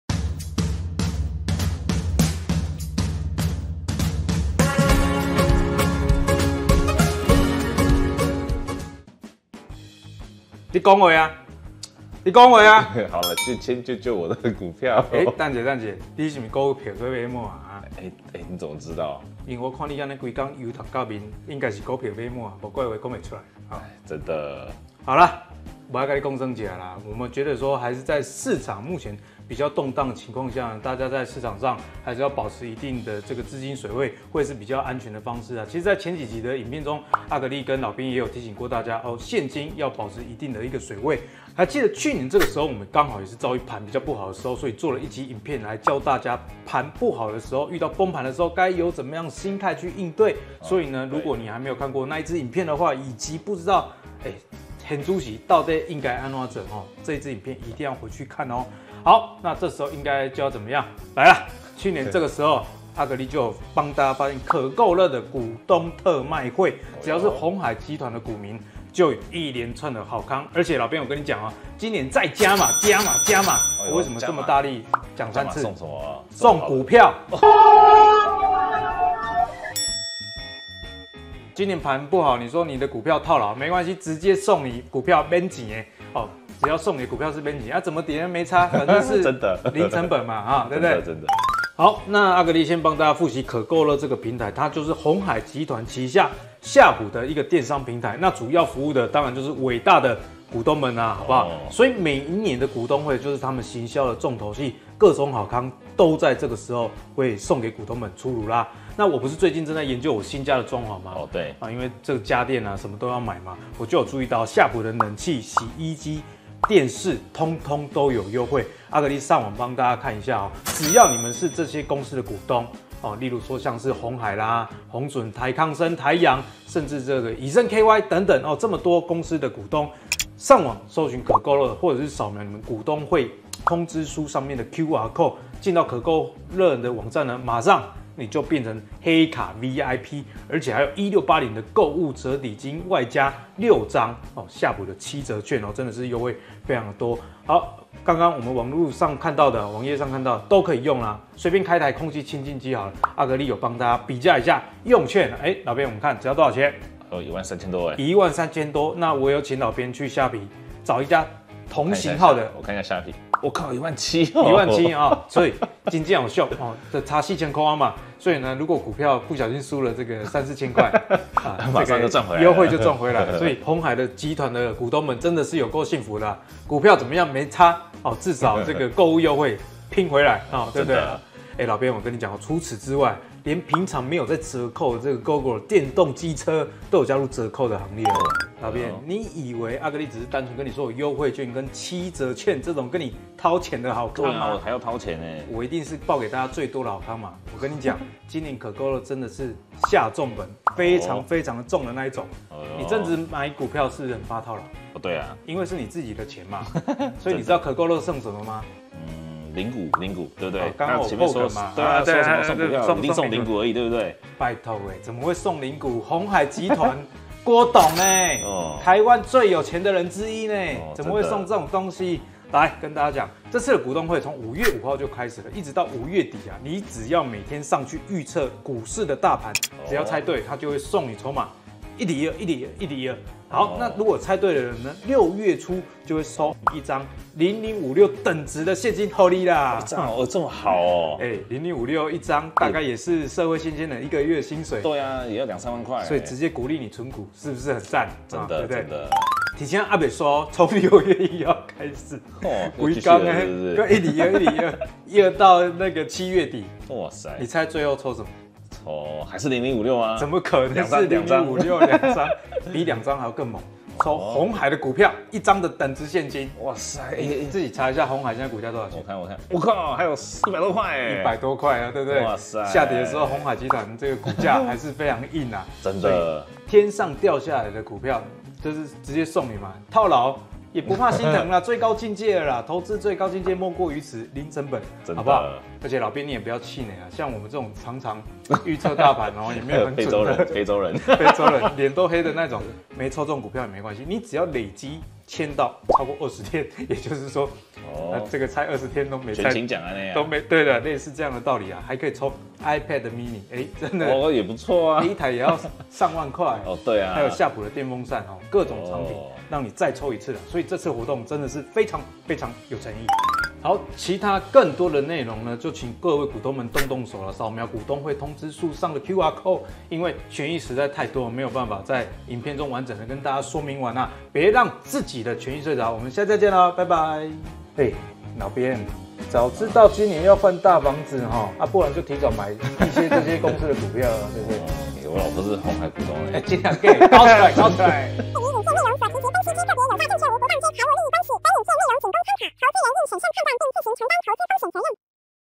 【阿格力】免費股票送給你，鴻海集團最狂股東福利，只怕你不搶！
🔔 大盤不好？沒關係！鴻海集團股東有福啦~~~鴻海集團旗下16家公司的股東們，只要掃瞄股條上的 QR Code 或是上網搜尋可購樂，登入即可領取黑卡VIP、1680購物折抵金以及6張夏普7折券！更重要的是，每天只要登入預測大盤漲跌，還能免費抽 0056 以及 鴻海 股票的等值現金喔！這麼多好康還不趕快來看看！別忘了自己的權益，趕快動手登入吧~

✔影片重點看這裡
00:00 鴻海集團股東福利又擱來啊，今年有夠大方！
01:36 什麼是可購樂？來來來，這麼多好康不要錯過
03:08 股東有夠爽，預測漲跌就抽0056跟鴻海股票的等值現金！
04:45 預測不準沒關係，全勤照樣抽 iPad mini 等好禮~